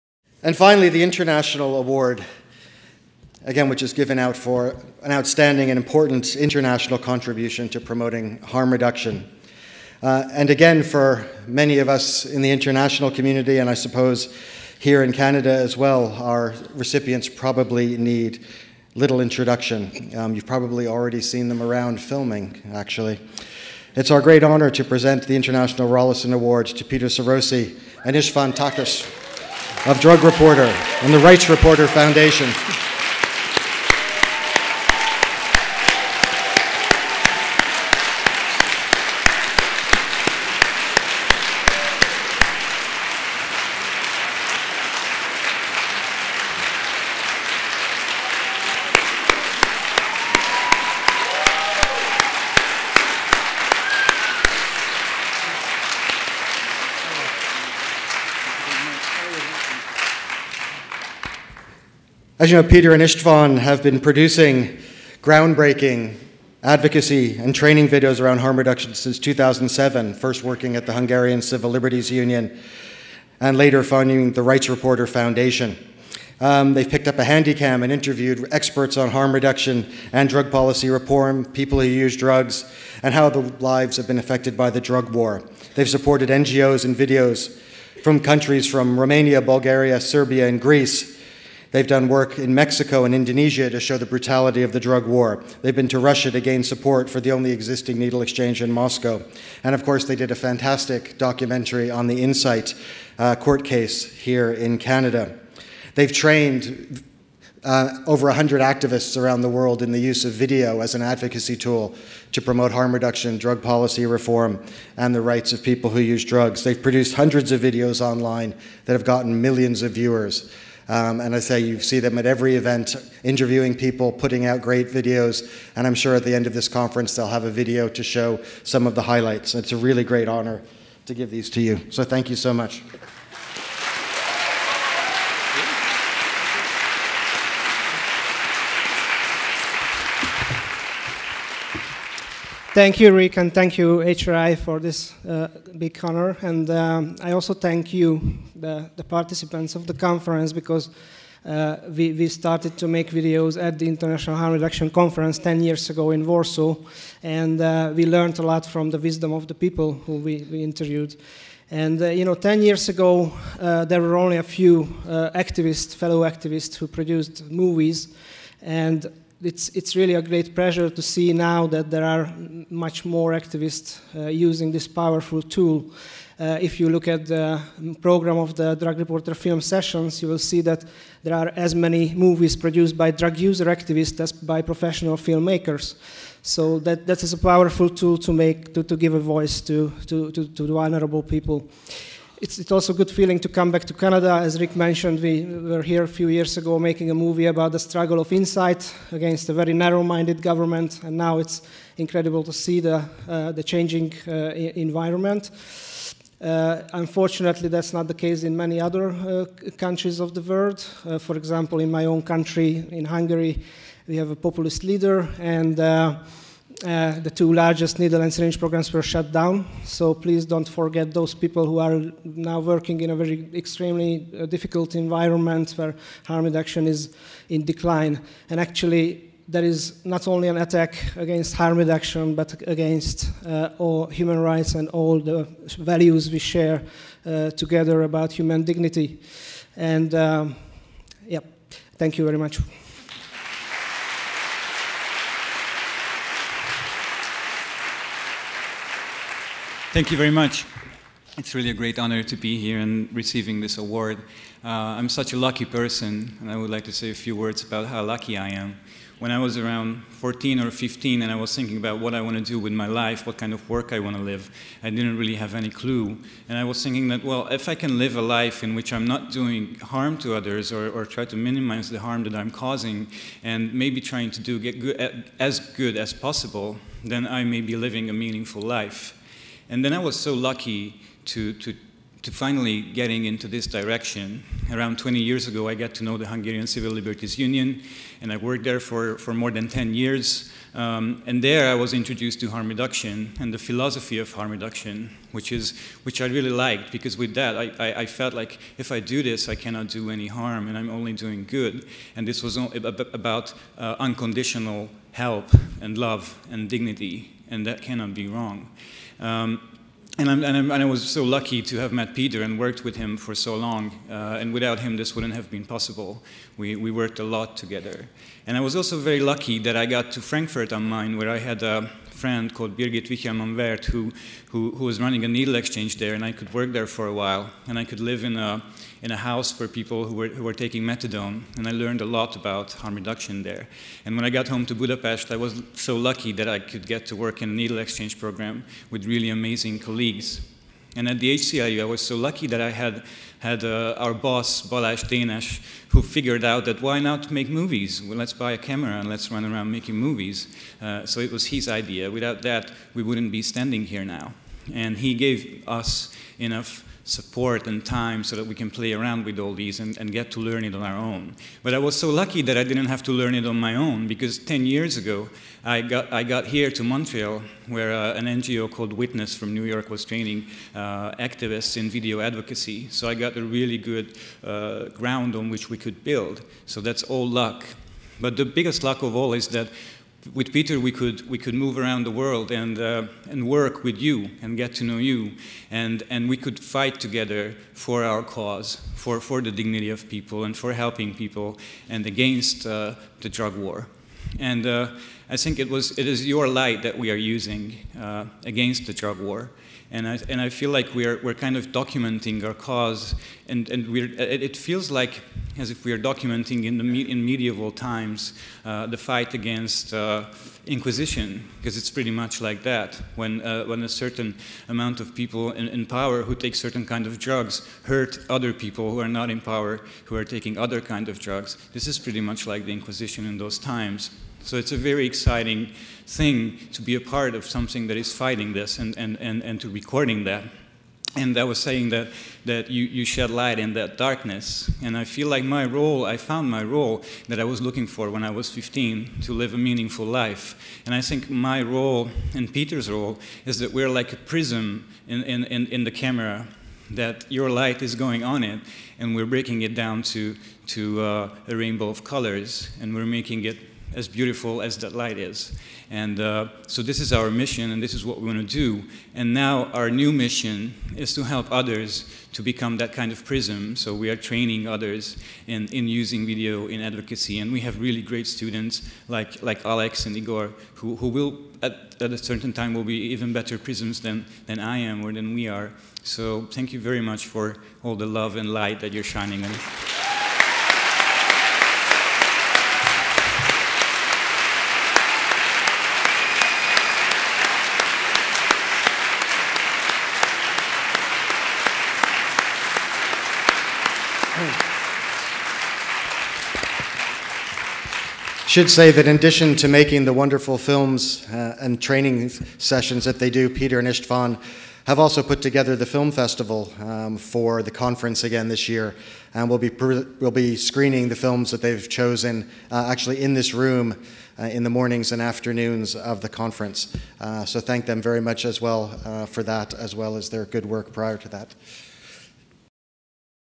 Drugreporter Team Receives The International Rolleston Award at HR17
Péter Sárosi and István Gábor Takács are honoured to receieve the most prestigous international award for harm reduction advocacy.